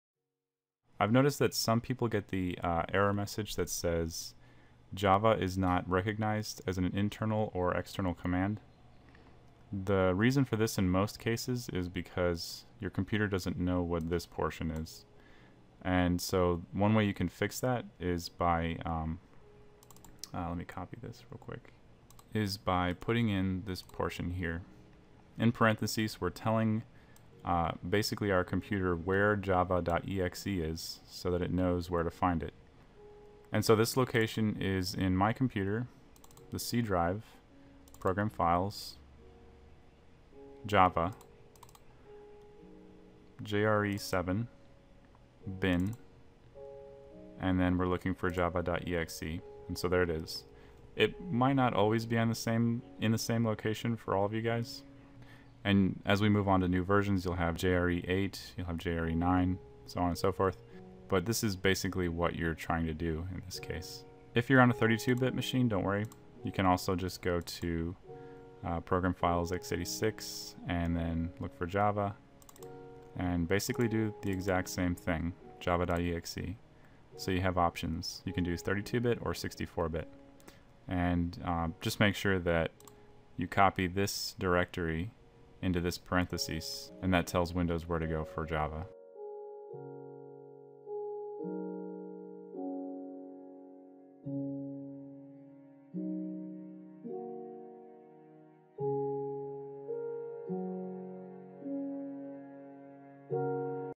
Minecraft: How to Fix the "java is Not Recognized" Error With Batch Files
How to fix the "java is not recognized as an internal command or external command" error in Windows.

▬▬▬▬▬▬ LINKS ▬▬▬▬▬
This is a snippet from a more in depth video on using batch files with Minecraft.

▬▬▬▬▬ RIGHTS ▬▬▬▬▬
Mojang owns the copyright to Minecraft: